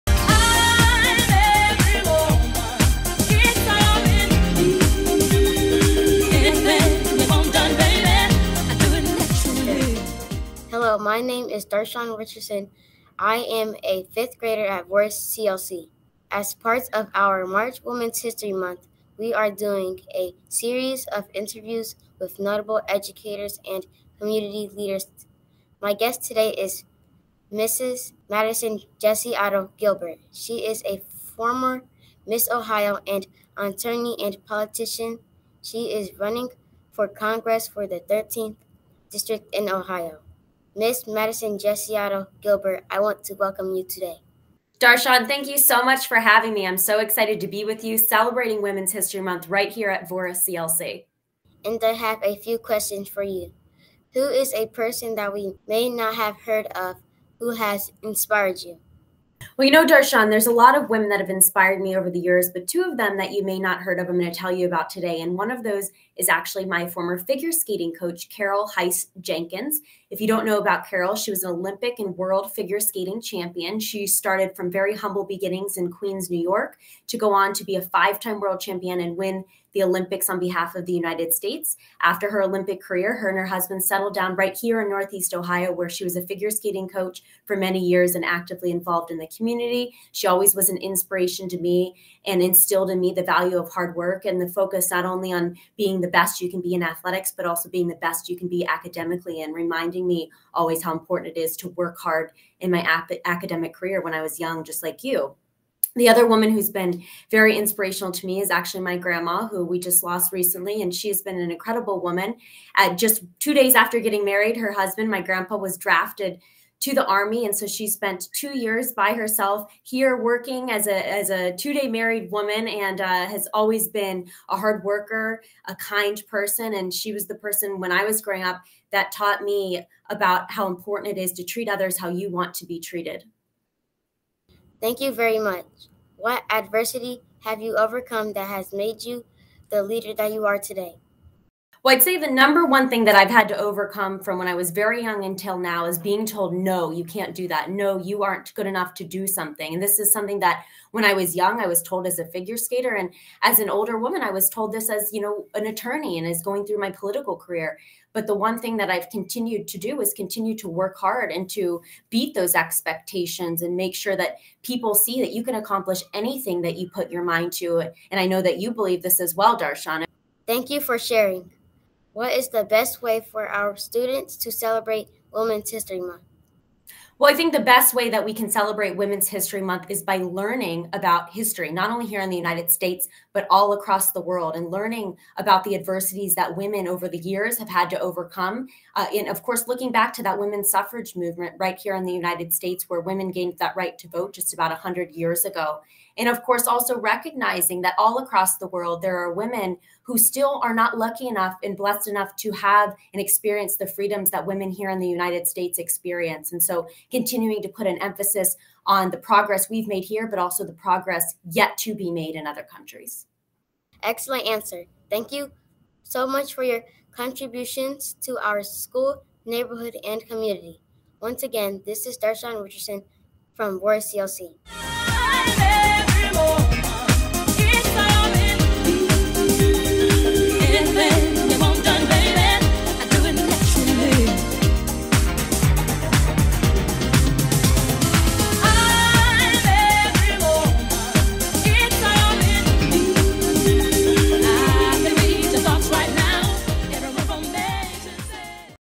Women's History Month Series - Interview with Mrs. Madison Gesiotto Gilbert
DarShaun is a 5th grade student at Voris CLC. As part of our Women's History Month video series, DarShaun interviewed the honorable Mrs. Madison Gesiotto Gilbert, attorney, politician and former Miss Ohio. Watch the video to hear more about her own history, the important roles women have played historically, and what she envisions women accomplishing in the future.

Big thanks to Rolando Pizana for his help with these features.